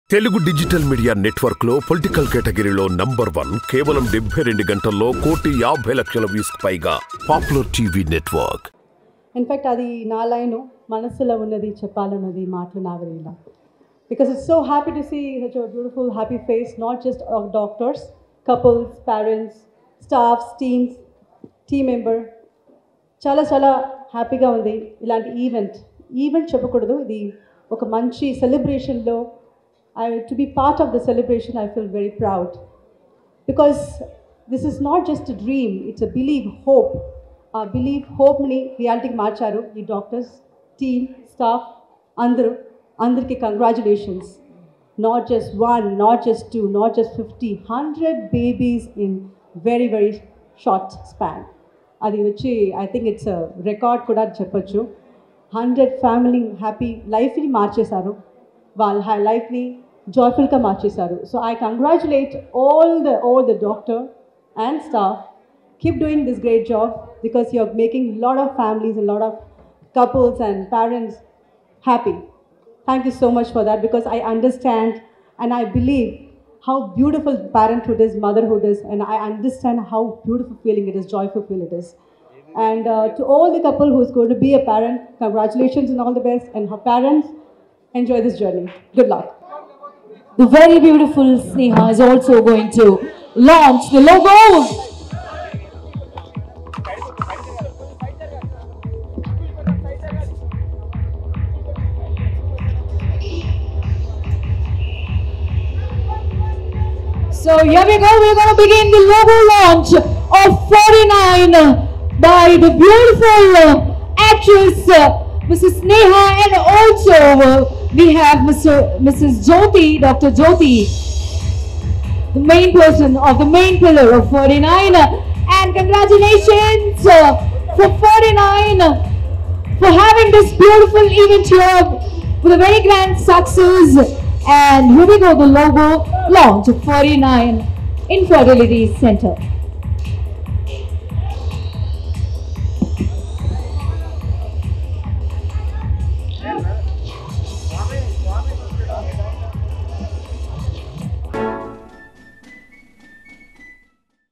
Actress Sneha Speech At Ferty9 Baby's Meet With The Launch of XILTRIX | Hyderabad |Telugu Popular TV
ఫెర్టీ 9 బేబీస్ మీట్ లో
హీరోయిన్ స్నేహ
క్యూట్ స్పీచ్